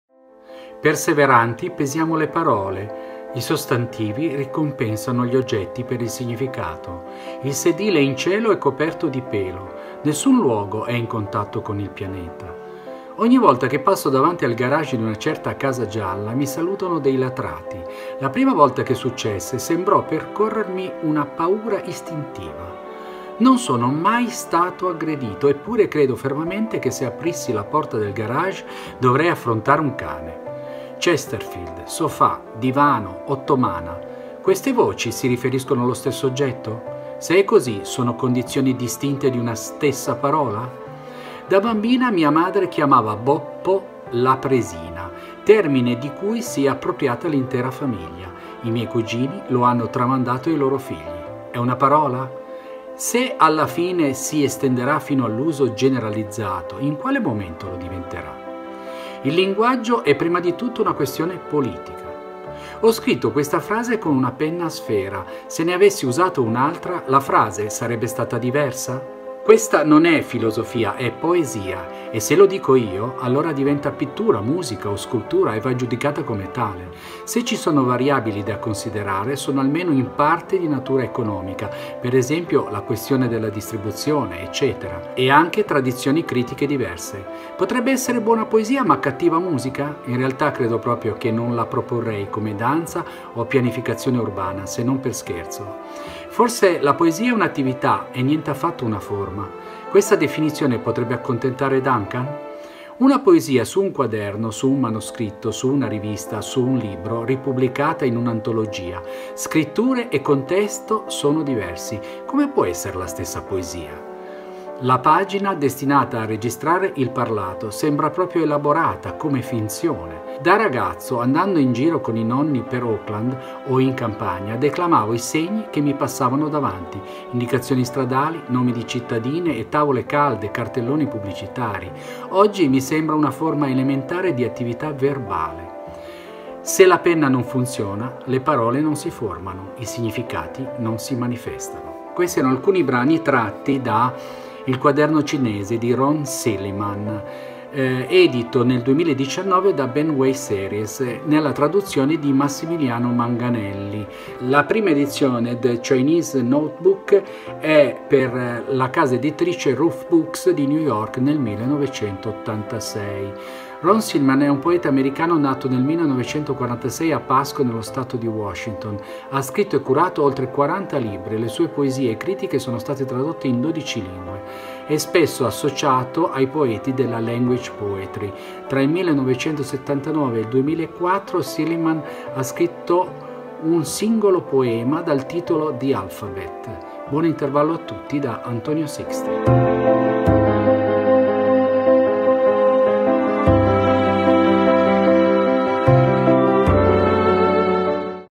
L' INTERVALLO DI ANTONIO SYXTY DEL 19 OTTOBRE - RON SILLIMAN
Ron Silliman (1946 Pasco, Washington, Stati Uniti) è un poeta americano. Ha scritto e curato oltre 40 libri e le sue poesie e critiche sono state tradotte in 12 lingue. È spesso associato alla language poetry. Tra il 1979 e il 2004, Silliman scrisse un singolo poema, The Alphabet.
He is often associated with language poetry. Between 1979 and 2004, Silliman wrote a single poem, The Alphabet. He has now begun writing a new poem, Universe, the first section of which appears to be called Revelator